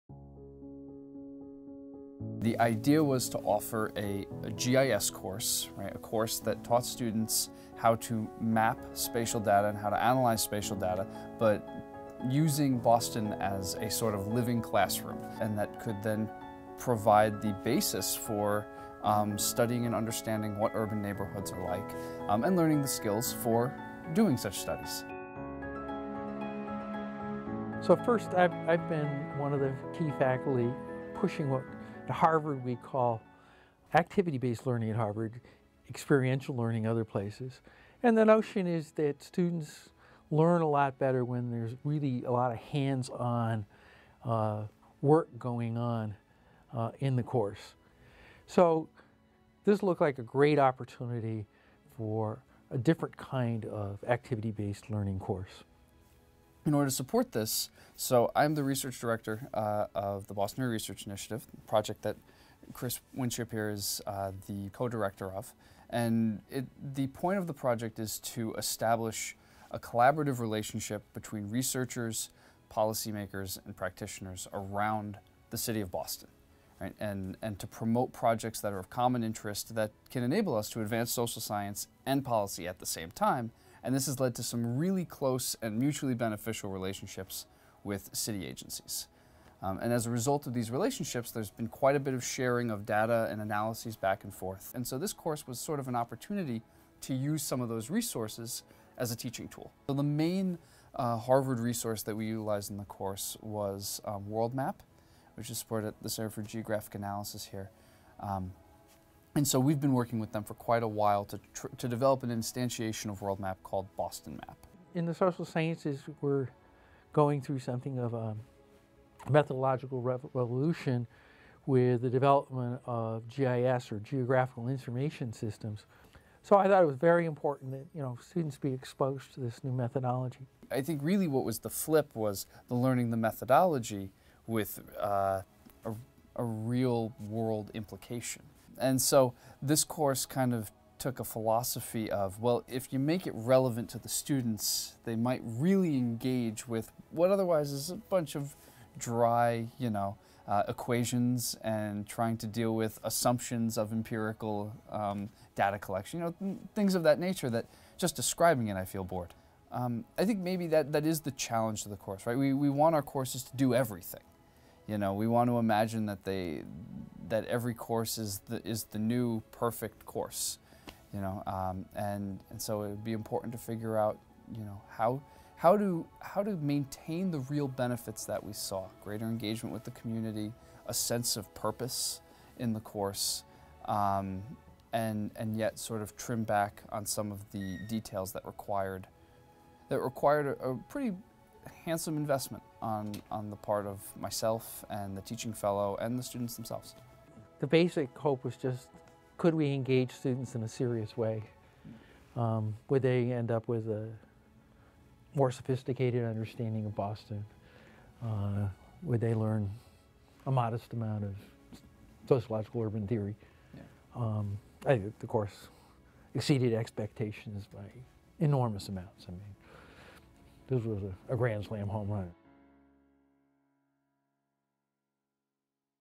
HILT 47 Christopher Winship and Dan O'Brien
Christopher Winship and Dan O'Brien discuss their Hauser Grant project (Exploring community differences using spatial data) where they created a new course that teaches students in sociology to visualize and analyze spatial data.